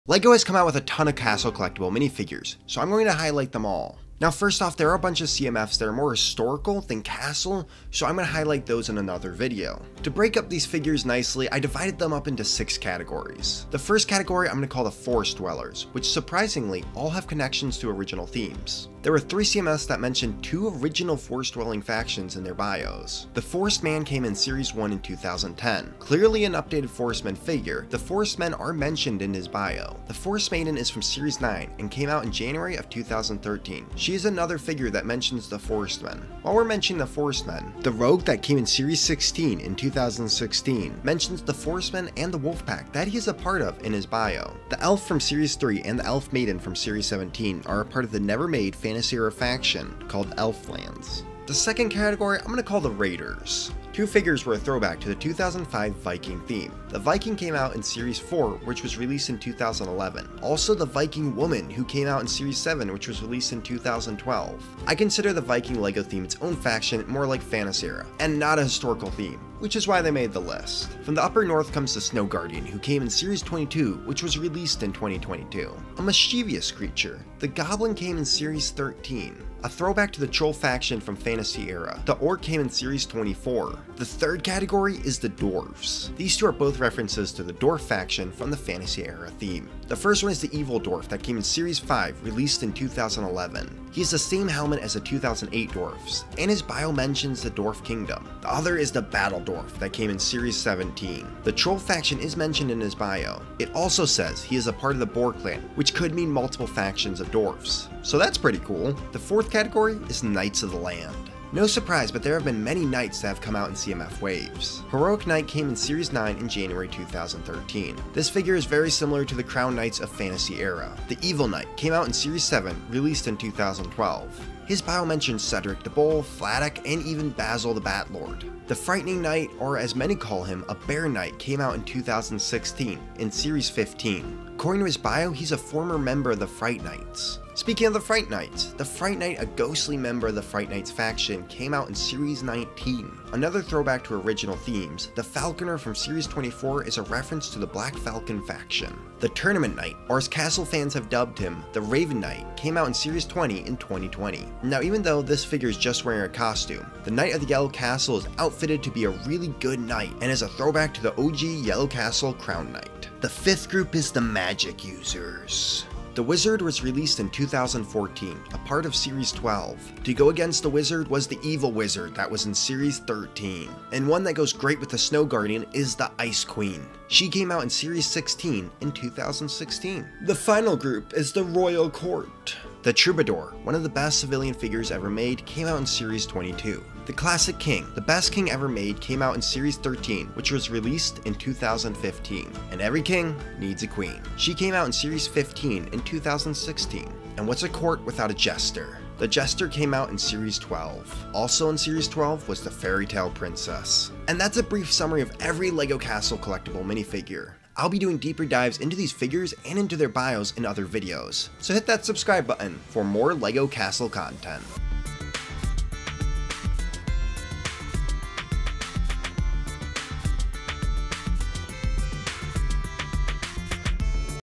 Every LEGO Castle CMF
Lego has come out with tons of castle collectable minifigures so lets quickly highlight them all. Now first off there have been many that are historical ones and I will cover that list in another video. Which one is your favorite?